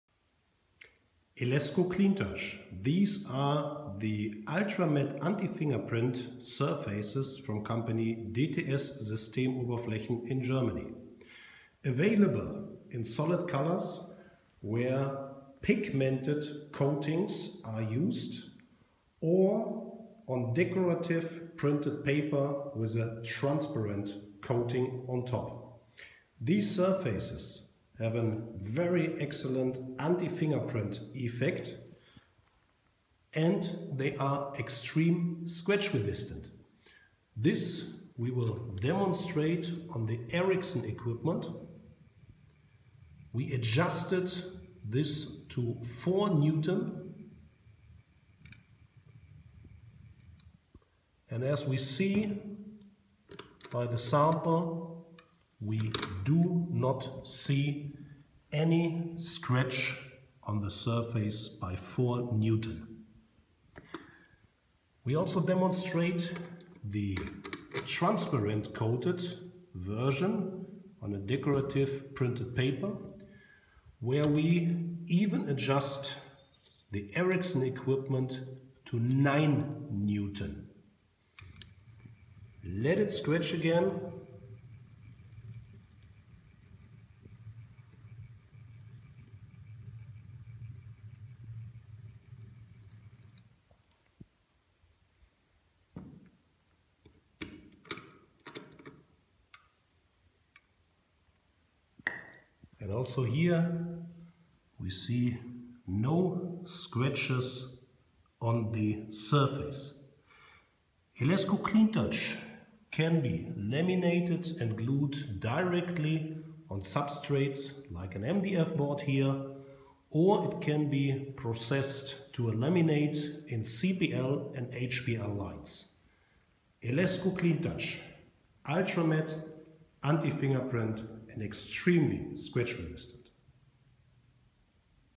Clean Touch scratch test
Erichsen scratch test on ELESGO Clean Touch, the extremely matt and anti fingerprint surface from DTS Systemoberflaechen GmbH, Germany